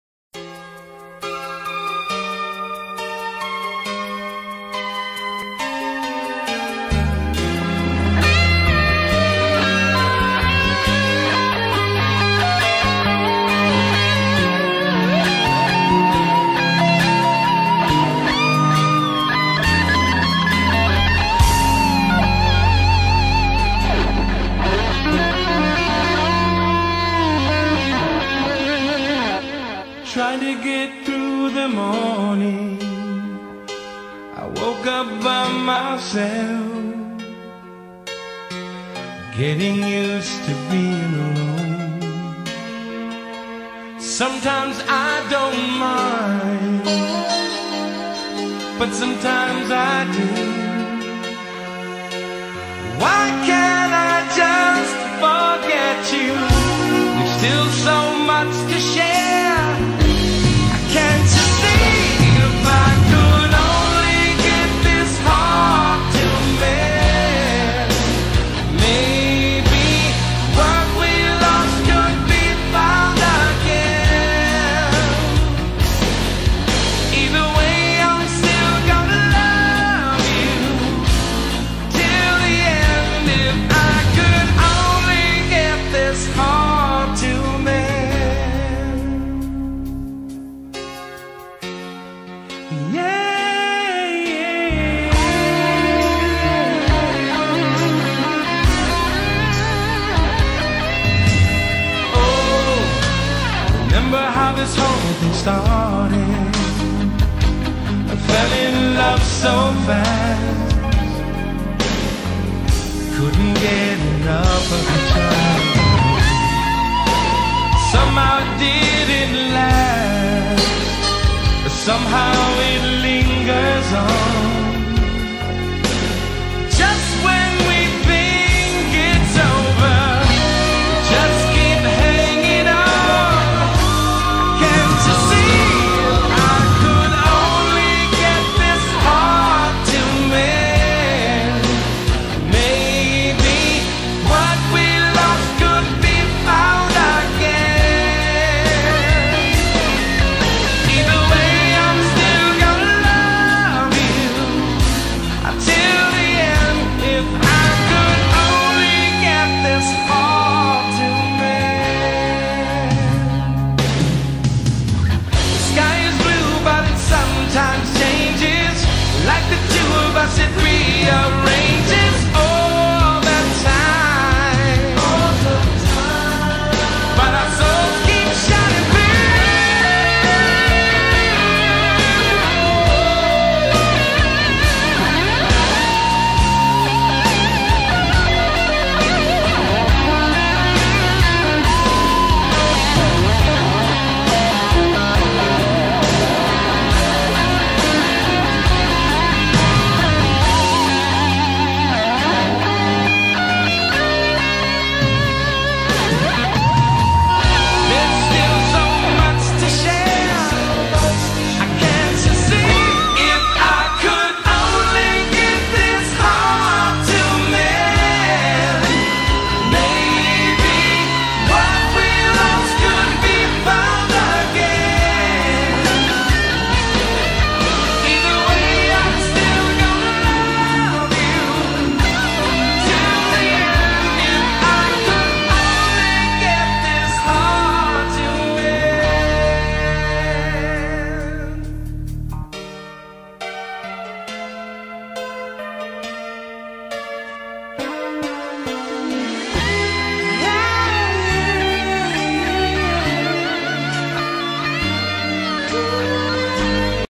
warren wiebe
the best singer of all time unfortunate death .This is another gem from the 80's with Warren Wiebe